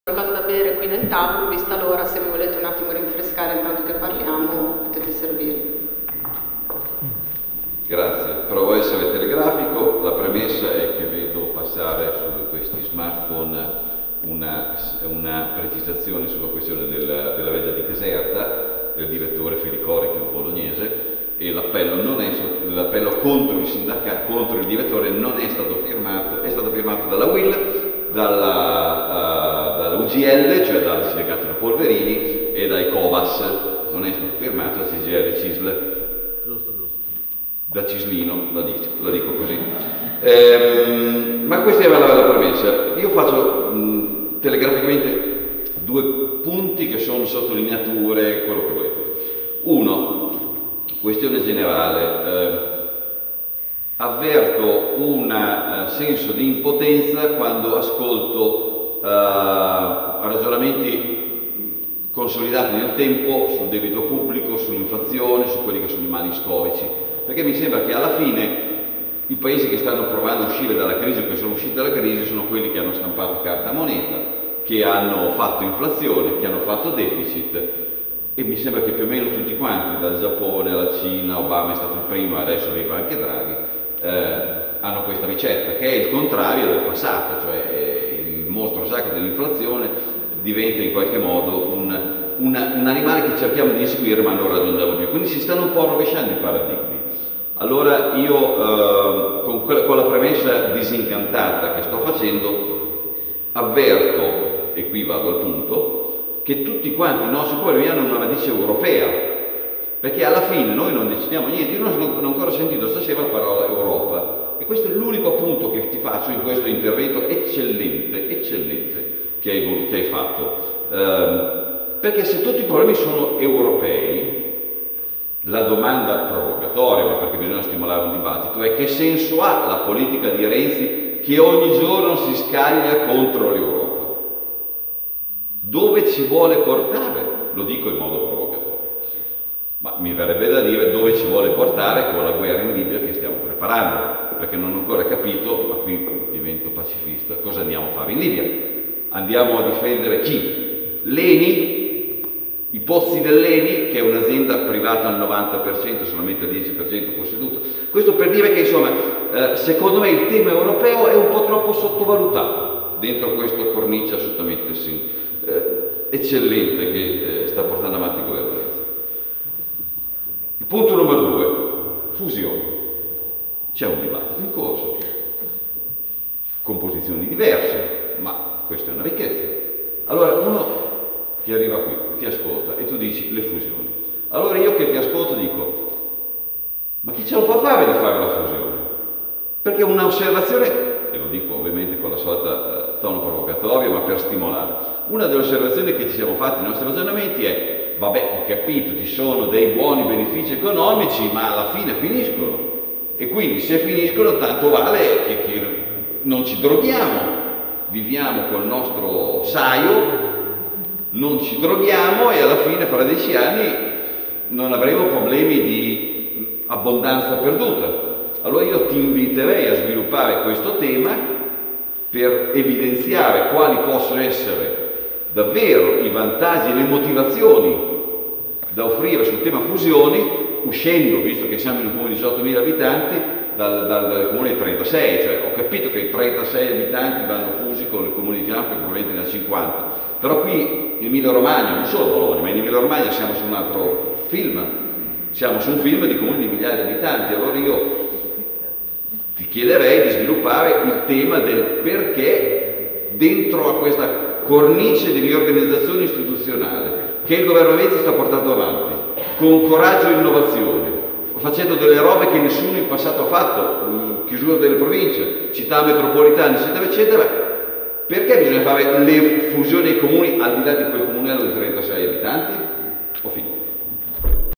7 Legge di stabilità Domanda Emanuele Burgin
Legge di stabilià Quali opportunità per cittadini e imprese?

Venerdì 4 marzo alle ore 20:30
Sala dell'Arengo Piazza della Repubblica n.1 Zola Predosa

Saluto introduttivo:
Marilena Melchiorri Segretaria Pd Zola Predosa

Modera:
Norma Bai Segretaria PD Zona Reno Lavino Samoggia

Relatore
Luigi Marattin Consigliere Economico del Presidente del Consiglio - Alma Mater Studiorum Università di Bologna